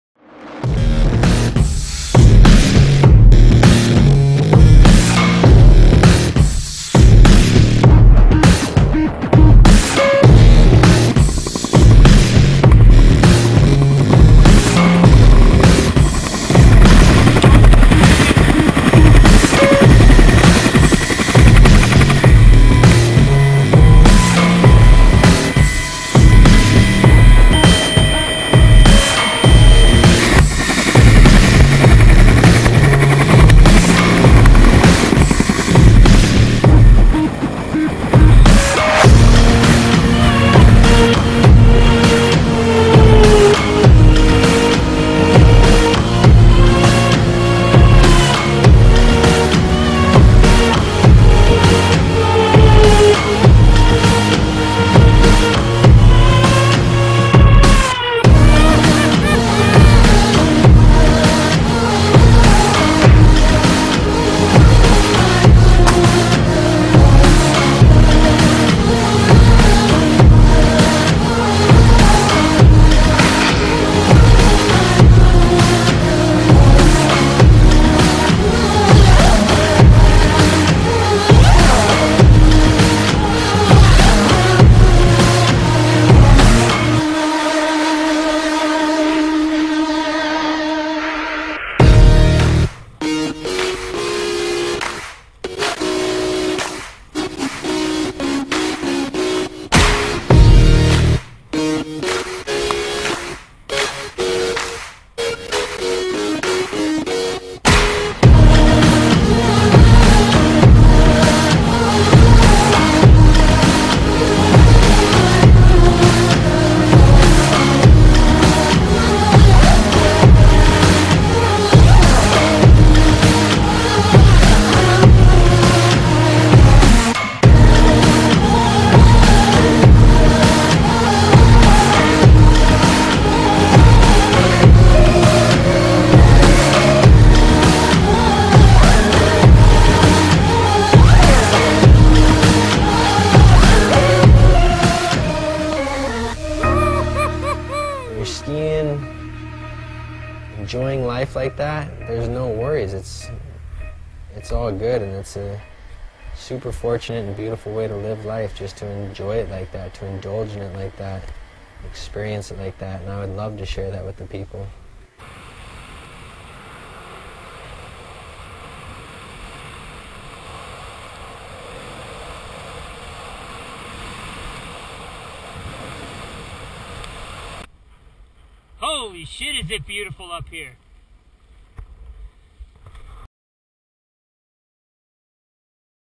CMH Heli Skiing
This is a short video i put together for someone I ride with.  Filmed with GoPro Hero 2 HD.  Location BC.